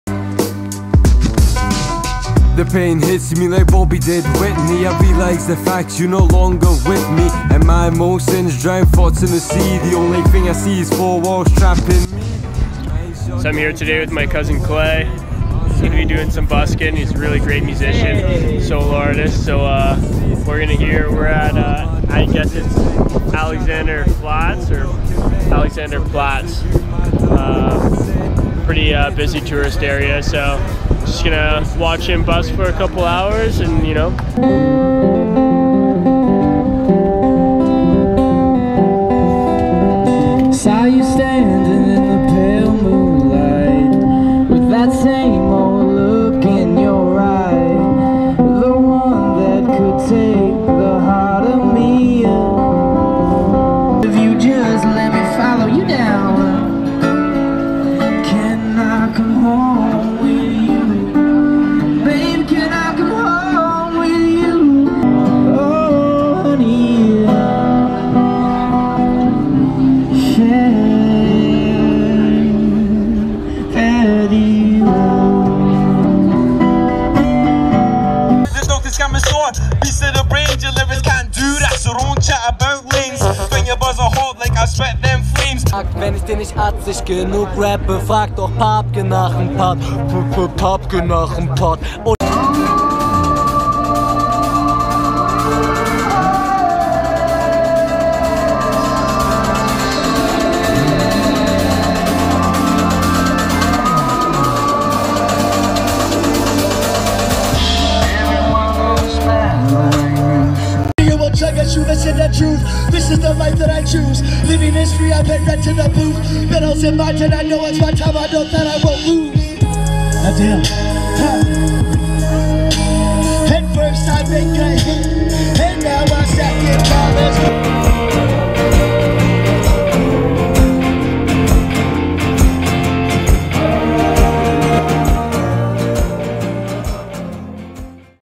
Berlin Music and Arts
Here you get a glimpse of the popular Kreuzberg district of Berlin Germany. Many street musicians and performers can be found here. As well as a lot of street art.